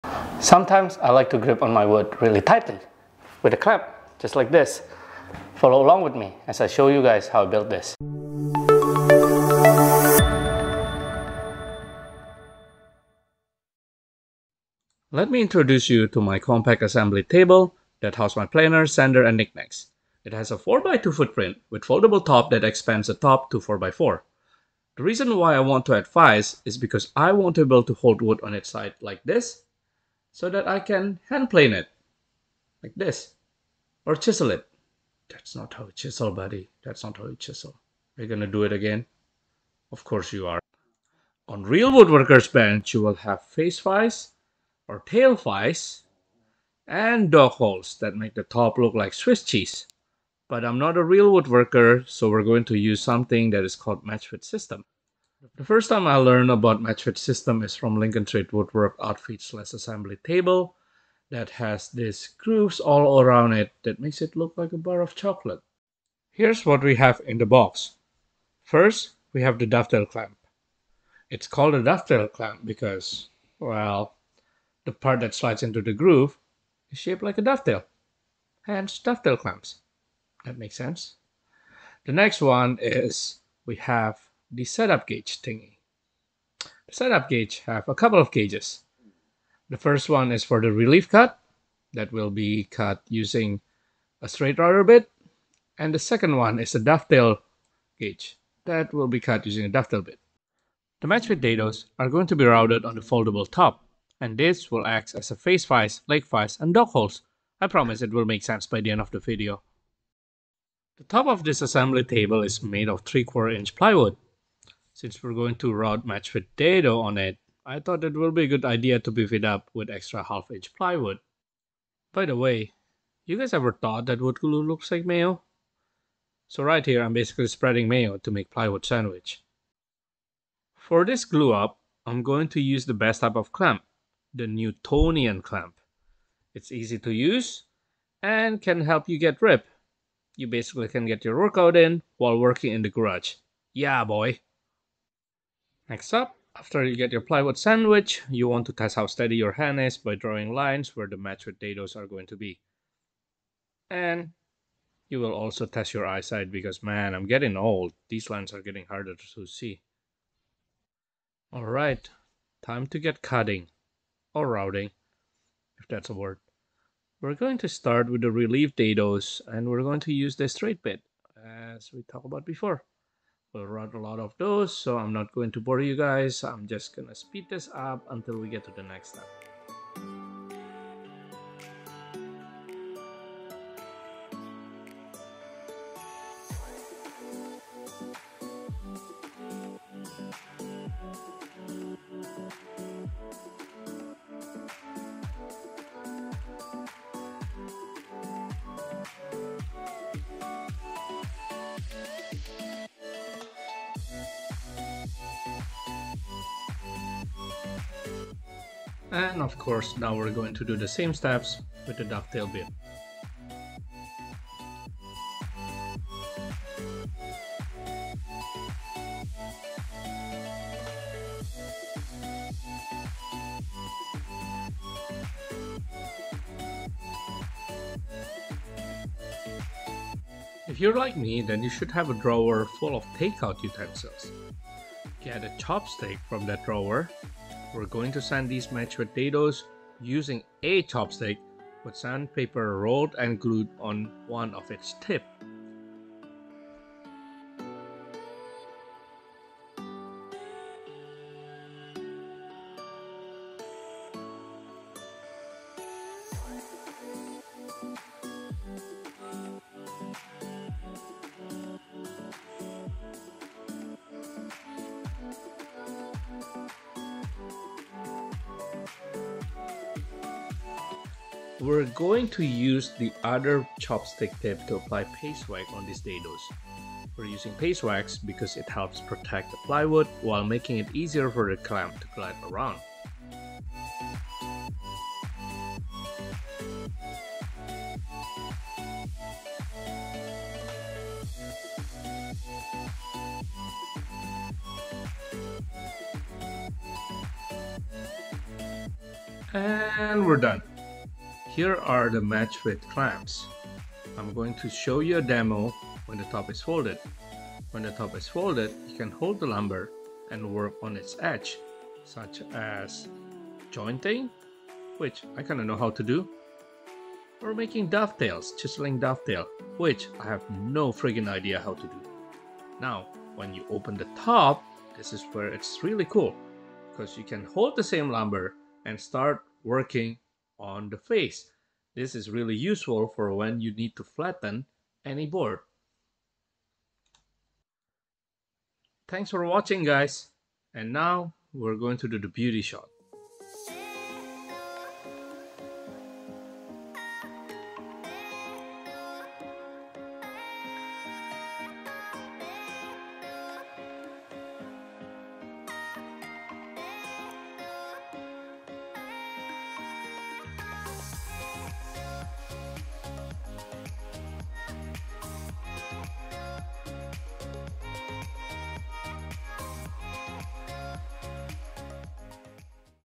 I did not expect this from the Microjig Matchfit || Workbench Improvement
Watch my workshop shenanigans! Adding Microjig Matchfit Vise to my assembly table and unintentionally added a feature I did not expect